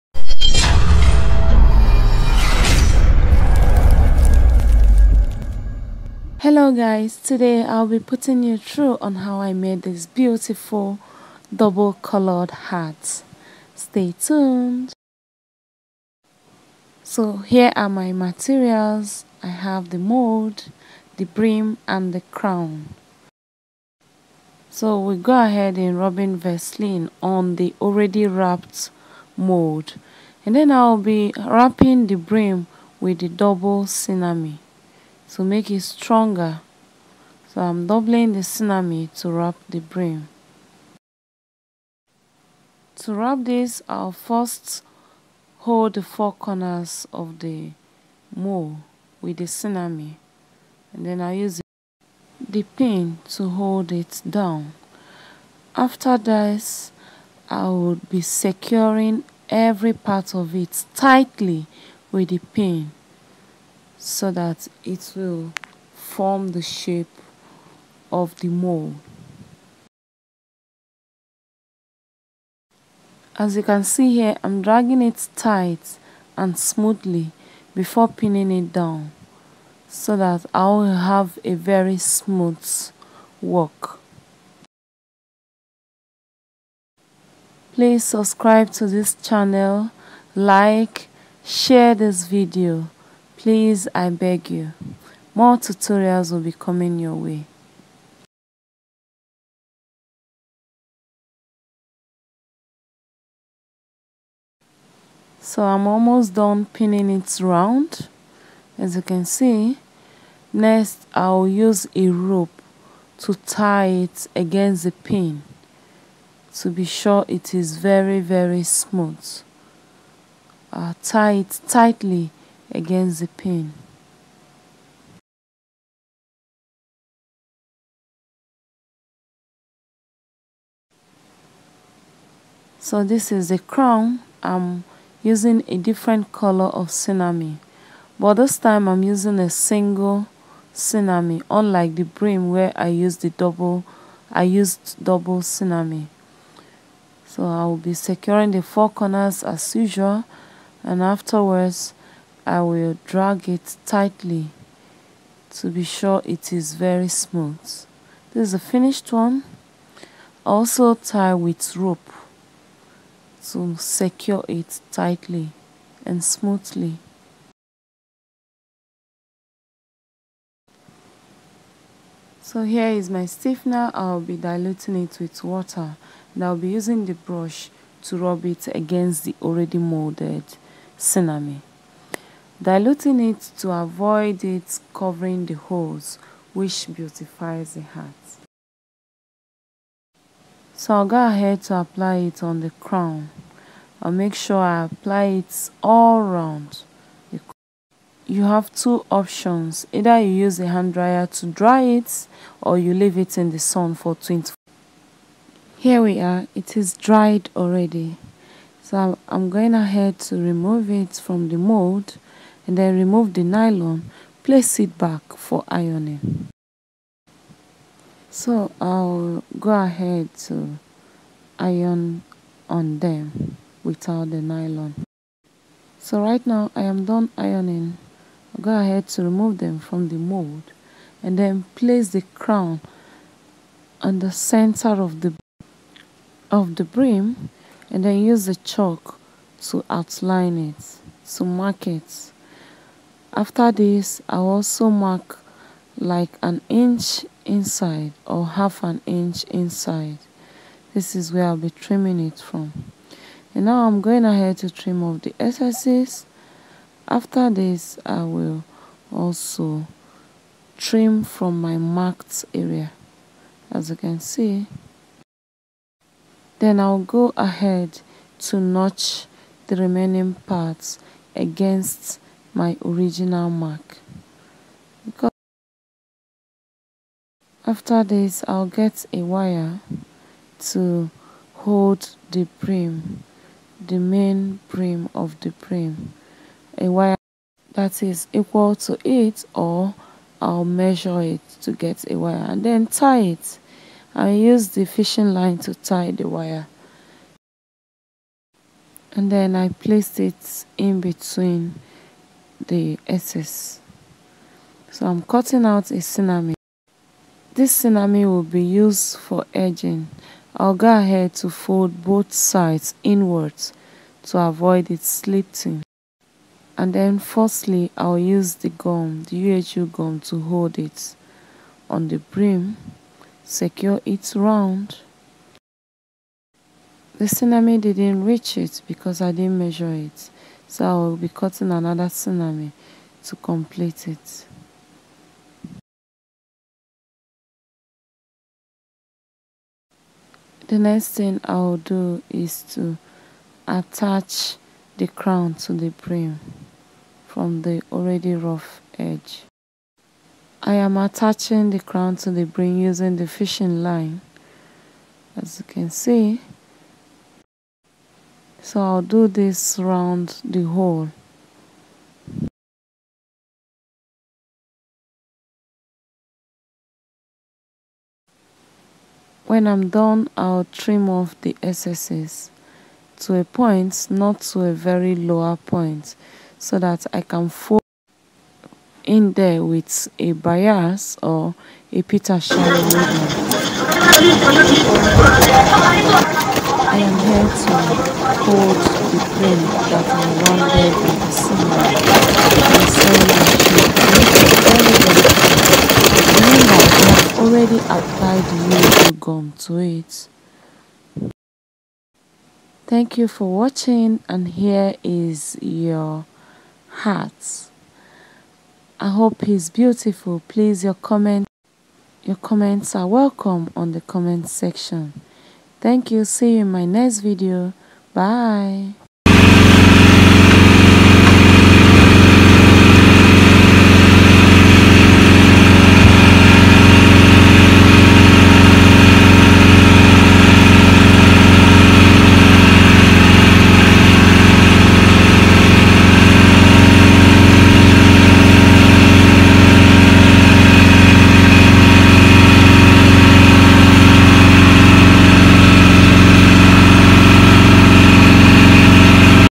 How to Mold A Hat Using Sinamey From Scratch to Finish
👉TURN ON MY POST NOTIFICATIONS FOR SHOUTOUTS AND AWARENESS